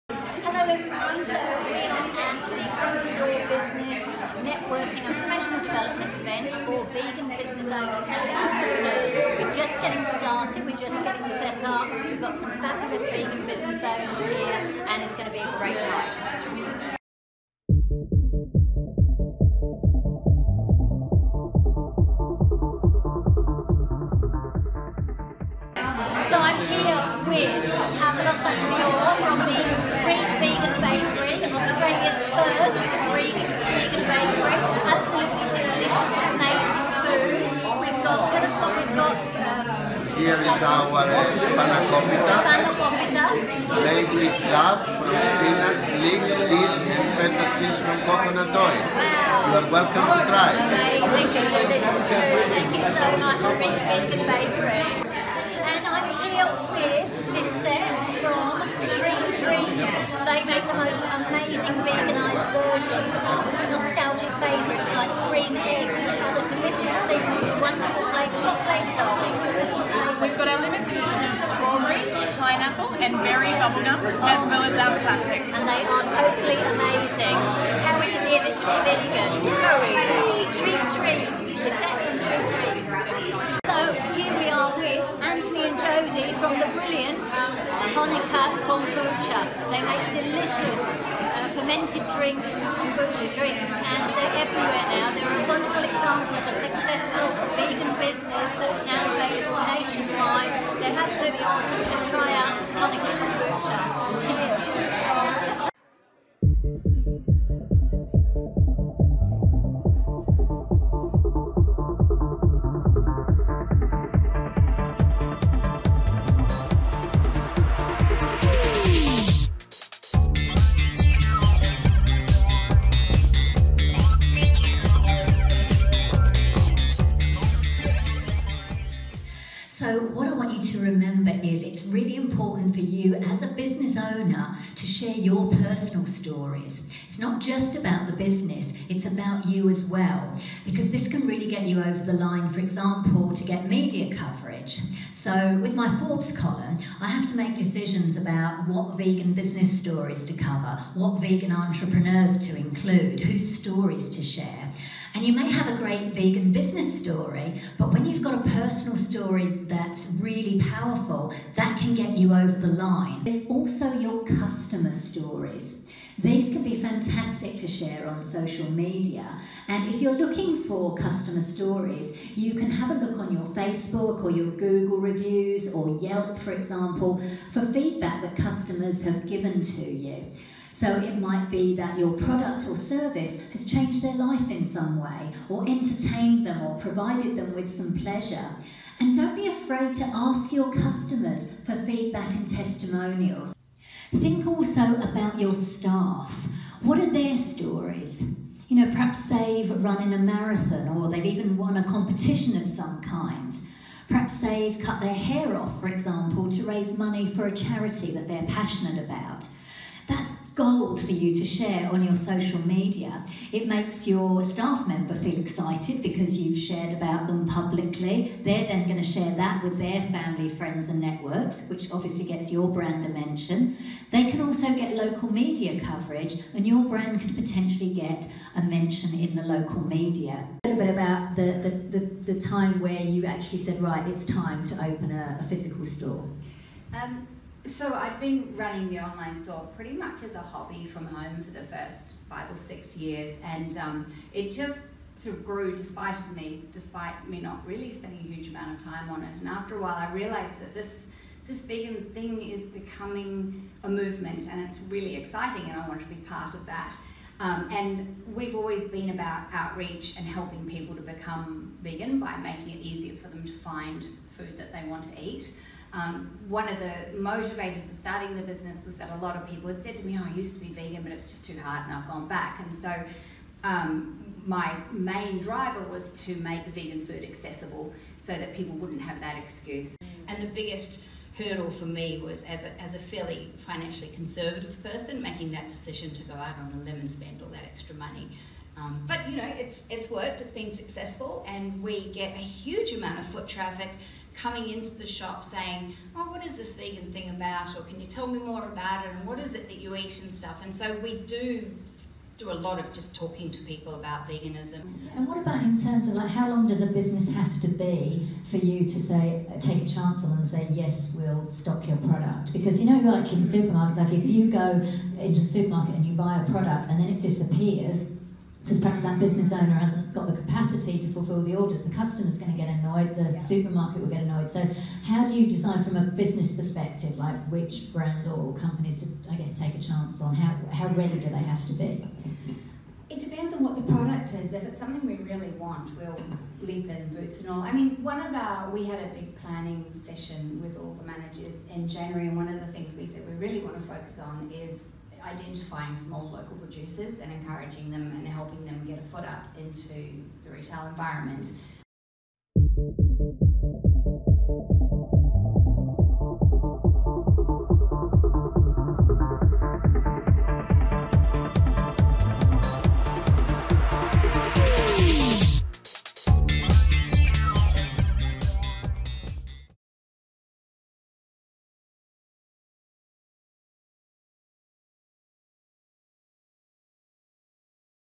Grow your vegan business networking & professional development event
Grow your vegan business is a networking and professional development event for vegan business owners and entrepreneurs in Sydney, Australia.

The most recent event took place on Wednesday 21 March. What a fabulous group of people who turned up to learn, share and support each other.

Huge thanks to our amazing food and drink sponsors Greek Vegan Bakery, Treat Dreams and Tonicka. And to the brilliant Jessica Bailey, founder of The Cruelty Free Shop chain of vegan supermarkets in Australia for sharing her insights in an audience Q&A.

To find out more, and to get free tips delivered directly to your inbox on how to grow your vegan business, sign up at: